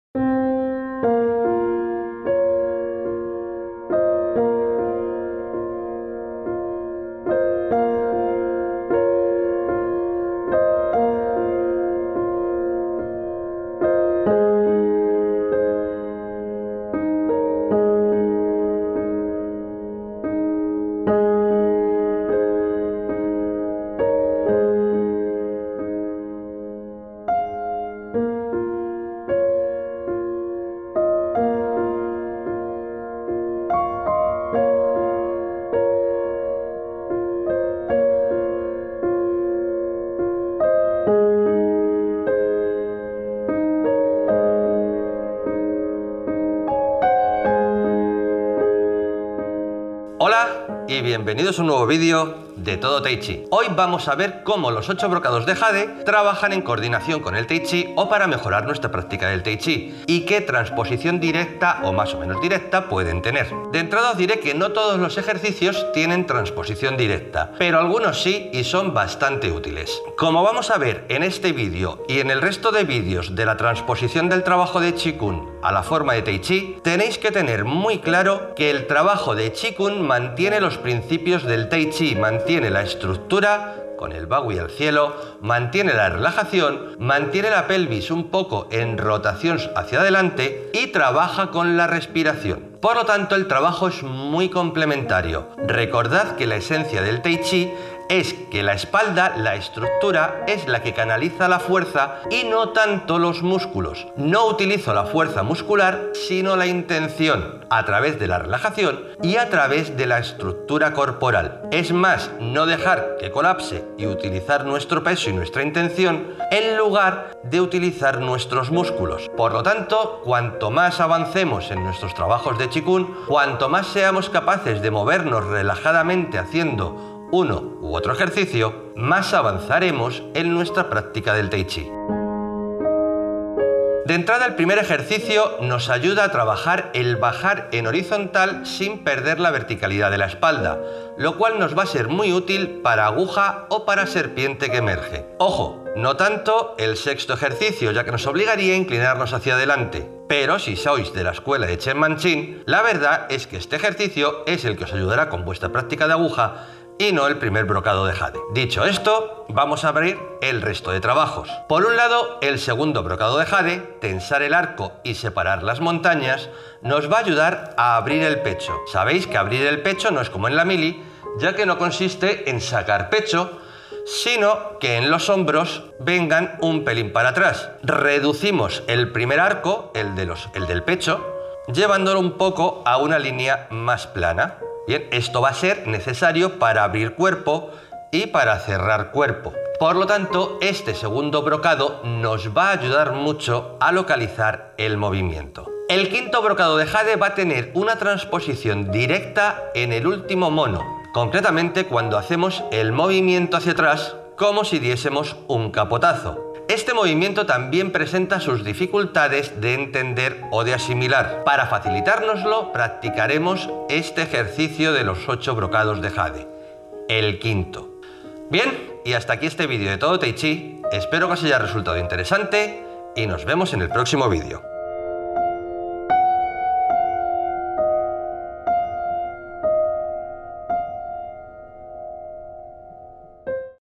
Del Baduanjin (8 brocados de jade) al Tai Chi
Seguimos con la serie de vídeos en los que profundizamos un poco más entre las relacciones del Tai Chi con los diferentes trabajos de Qi Gong. También veremos como practicar habitualmente estos ejercicios puede ayudarnos pulir algunos de nuestros movimientos.

Espero que os guste y que os resulte interesante.